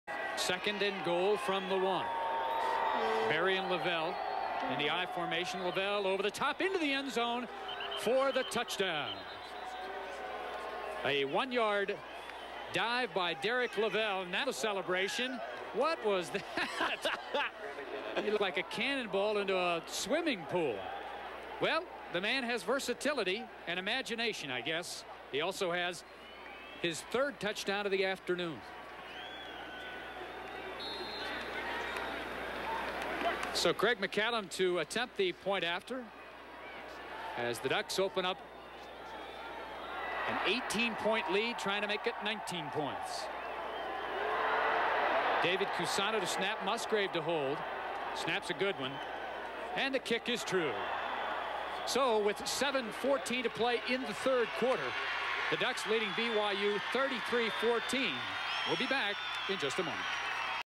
Oregon RB Derek Loville 1 yard touchdown run vs. BYU 11-04-1989
Oregon RB Derek Loville runs for a 1 yard touchdown vs. BYU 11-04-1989 at Cougars Stadium in Provo, UT